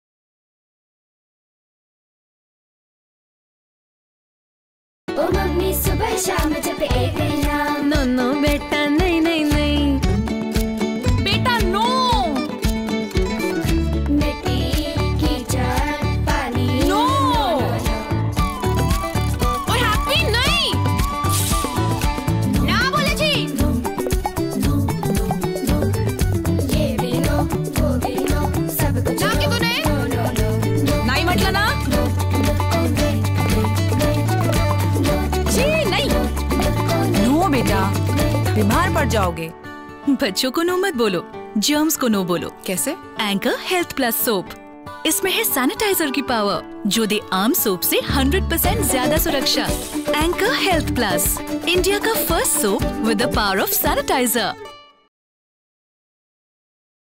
Anchor Soap | NO Film | Manoj Pillai | Arjuna Harjai | Surabhi Dashputra | Chrome Pictures
Anchor Soap
Director : Manoj Pillai
Music Director : Arjuna Harjai
Singer : Surabhi Dashputra
Mix Engineer : Anu Poojari